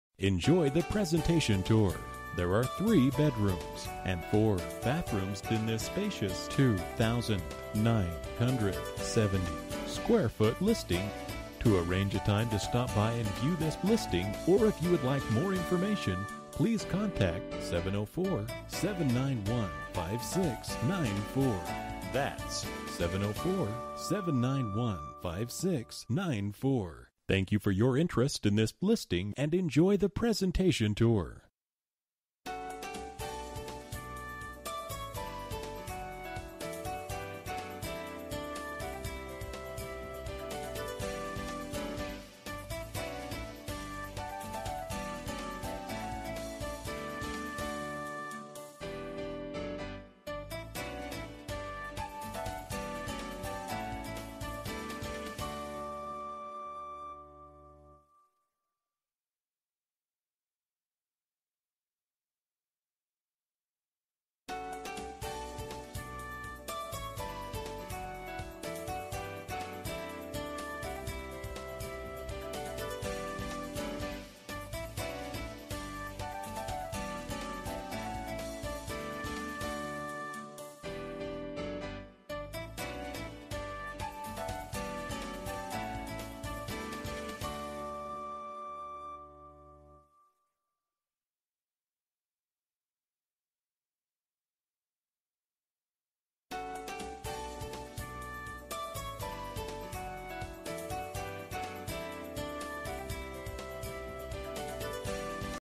Residential for sale - 1155 Cannon Farm Road, China Grove, NC 28023
Beautiful mini horse farm! 1.5 story home w/ full wrap around porch & basement! Approx 4.7 Acres! Large private pond, Fenced pasture, 4 stall barn, Beautiful master suite on main floor w/ elegant bath w/ Large walk in shower and his & her closet.  Tons of updates! Cherry Cabinets w/ granite tops. Stainless appl, breakfast bar, Large breakfast nook.  large bedrooms upstairs w/ Jack n Jill bath & finished basement w/ full kitchen, rec room & office. Brand new Hardwood floors in great room!

Bedrooms: 3
Bathrooms: 4 bathrooms
Square Feet: 2970
Price: $389,900

07/27/2014 06:12:10 pm

Last modified: